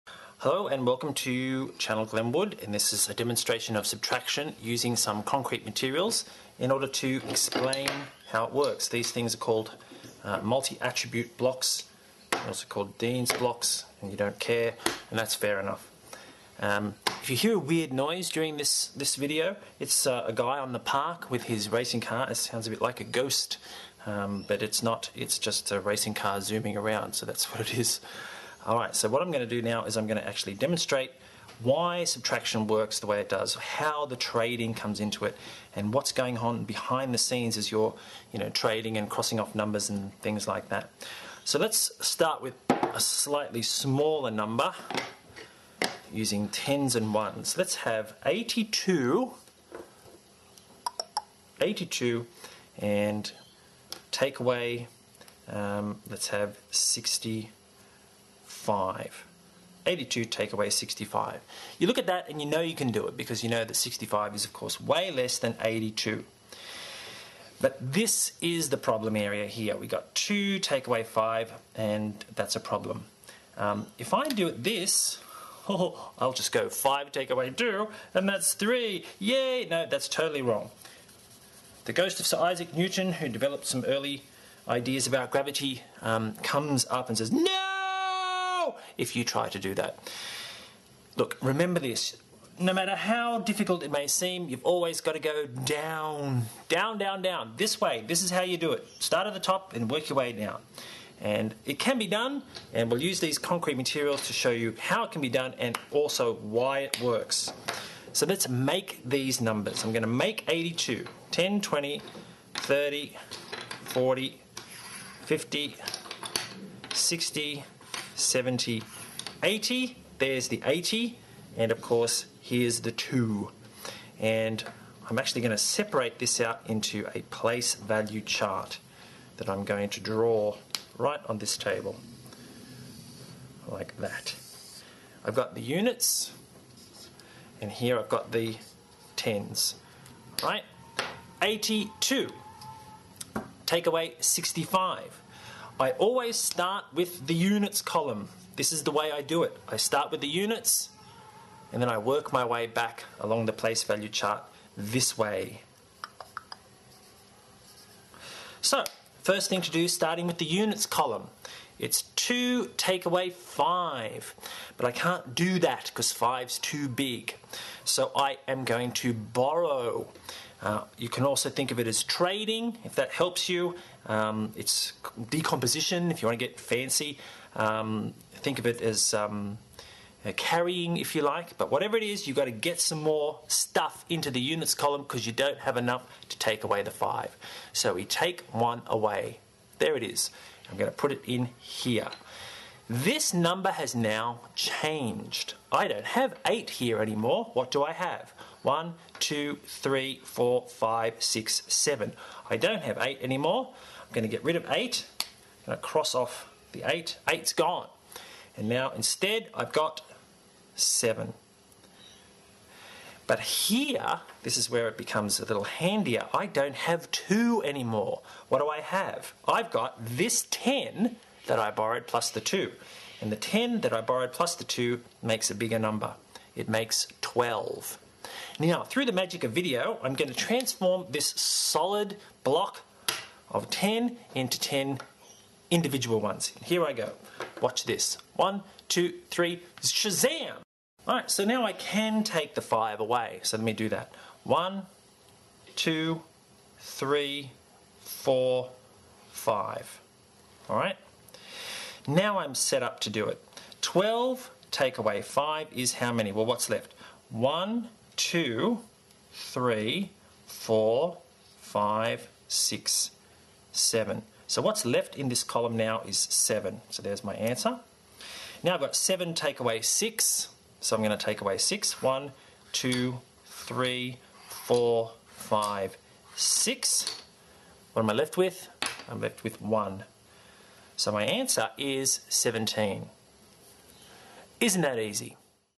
Subtraction with Blocks Part 1
Subtraction with trading (or borrowing or decomposition) using MAB (Dienes) blocks to explain how it works.